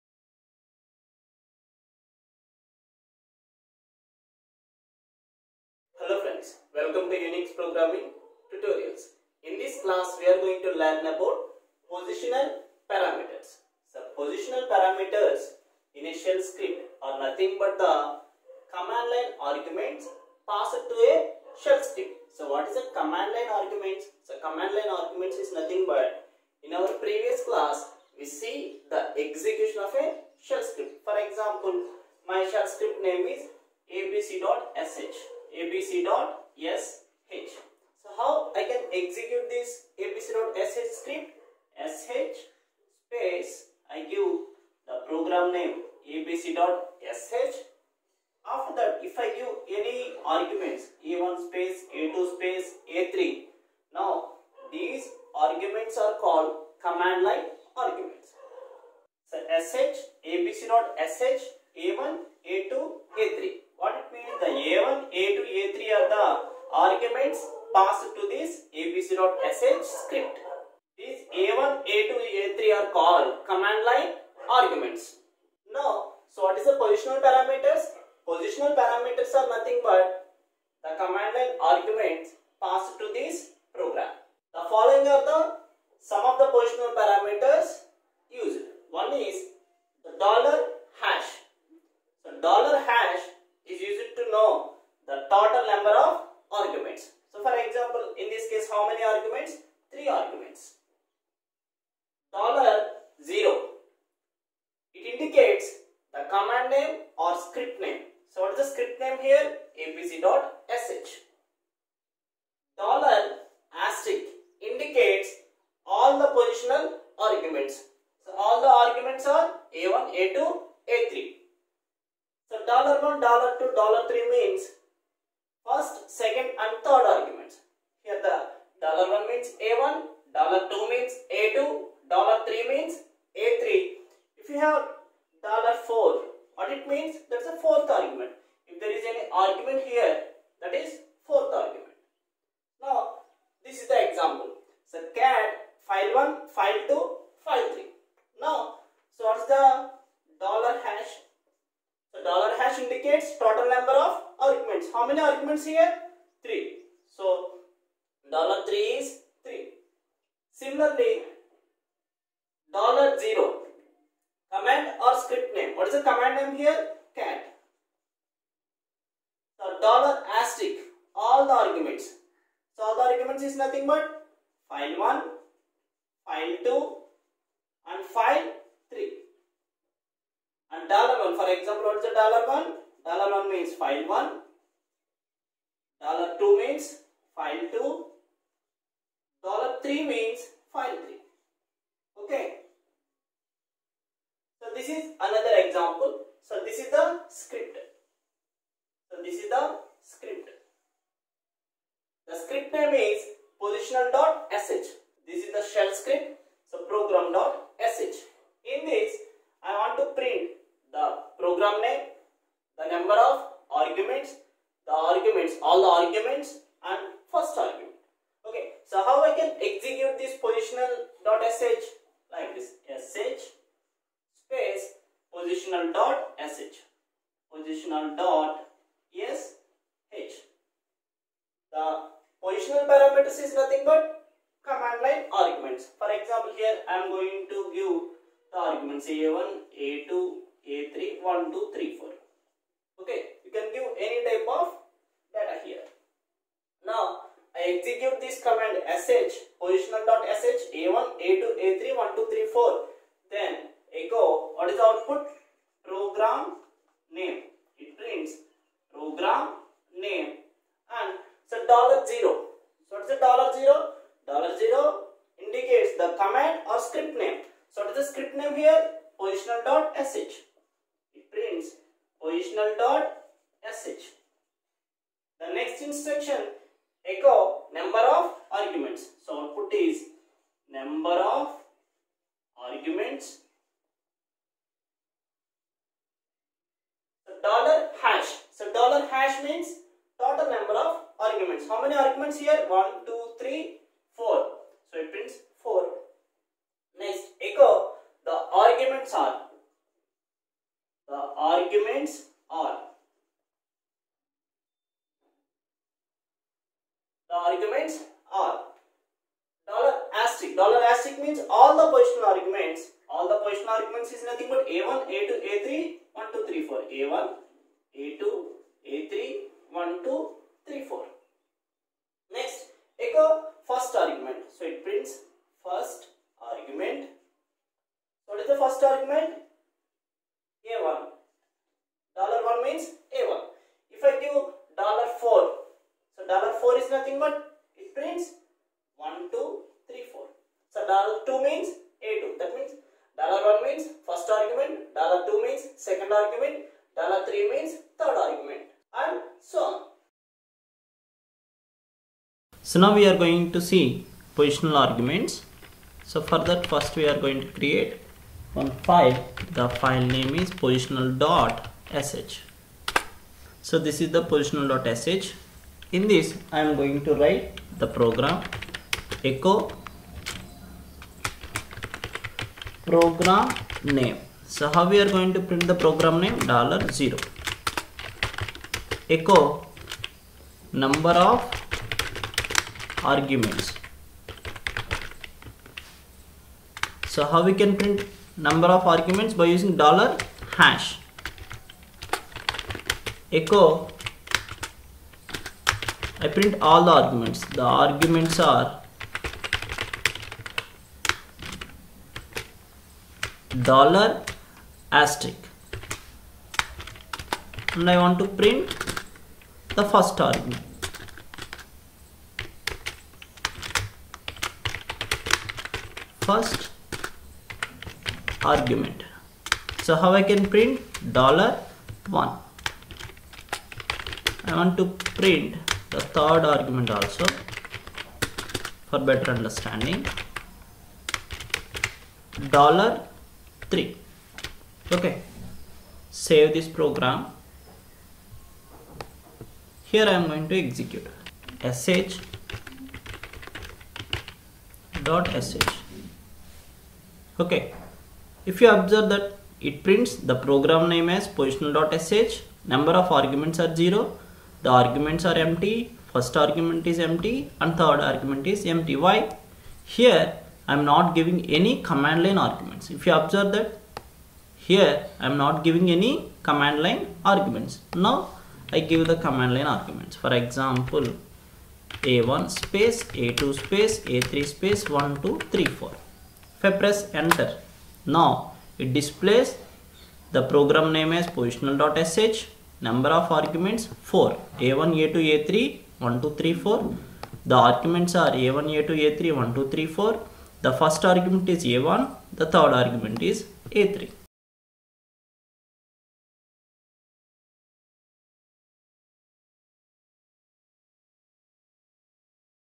UNIX Programming (Part - 45) Shell Programming (Positional parameters)
As Per JNTU - K ( R16) Syllabus

UNIT-V
Shell Programming-Shell Variables-The Export Command-The Profile File a Script Run During Starting-The First Shell Script-The read Command-Positional parameters-The $? Variable knowing the exit Status-More about the Set Command-The Exit Command-Branching Control Structures-Loop Control Structures-The Continue and Break Statement-The Expr Command:
Performing Integer Arithmetic-Real Arithmetic in Shell Programs-The here Document()-The Sleep Command-Debugging Scripts-The Script Command-The Eval Command-The Exec Command.